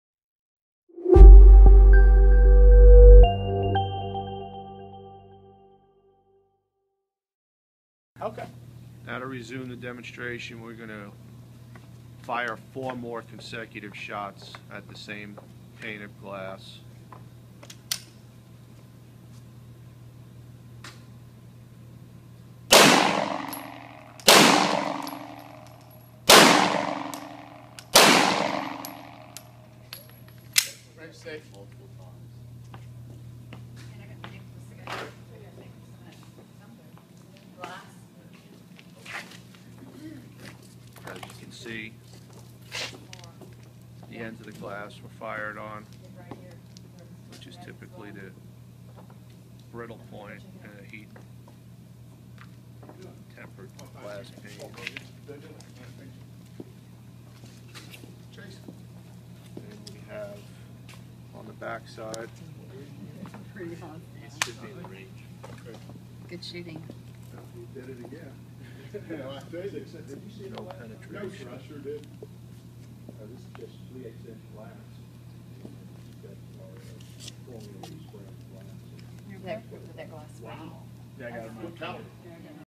Window Film Security Demonstration
Window film is shot several times and holds strong. Protect yourself now.